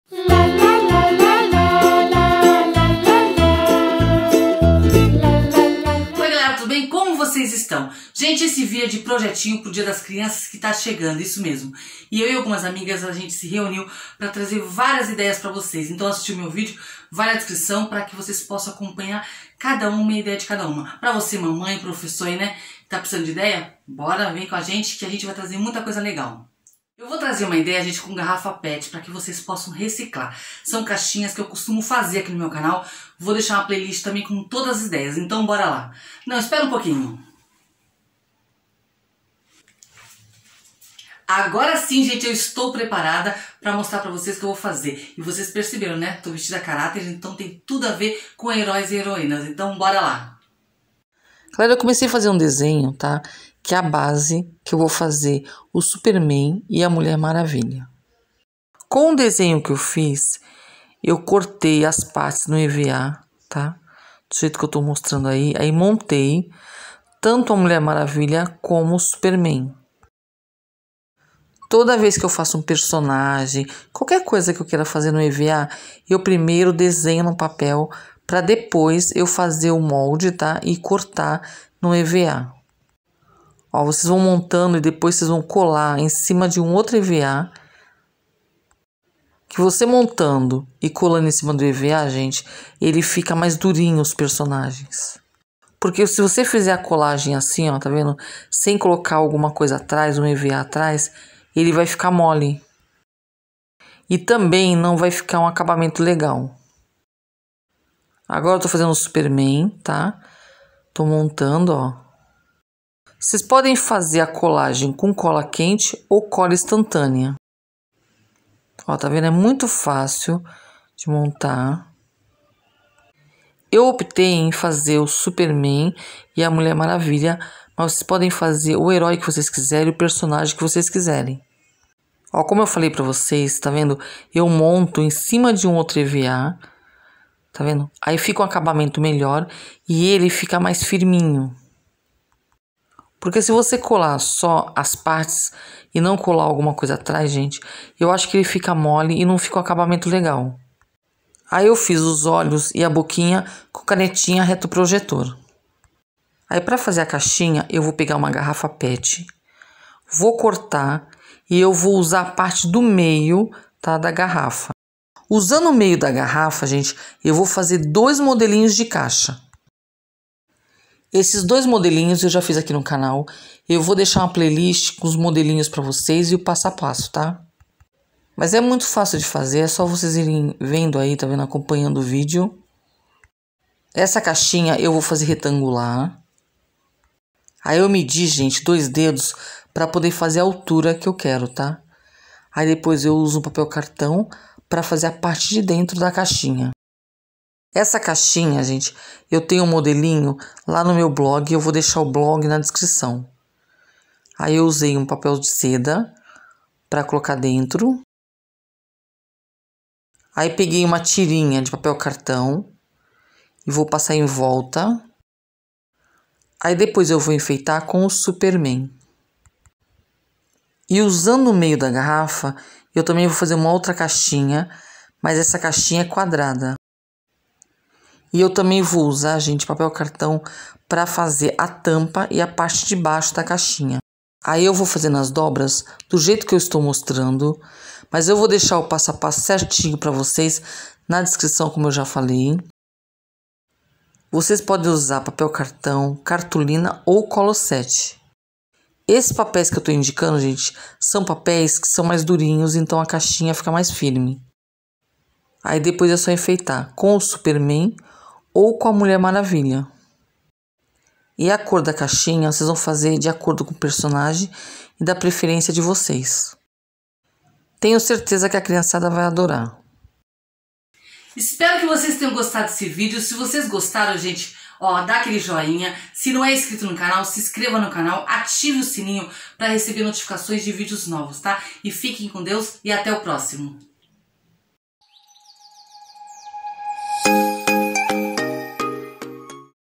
2 ideias para o dia das crianças
Nesse vídeo mostro como fazer 2 ideias para o dia das crianças

Participantes do projeto

*PROJETO DIA DAS CRIANÇAS*

Playlist caixas garrafa pet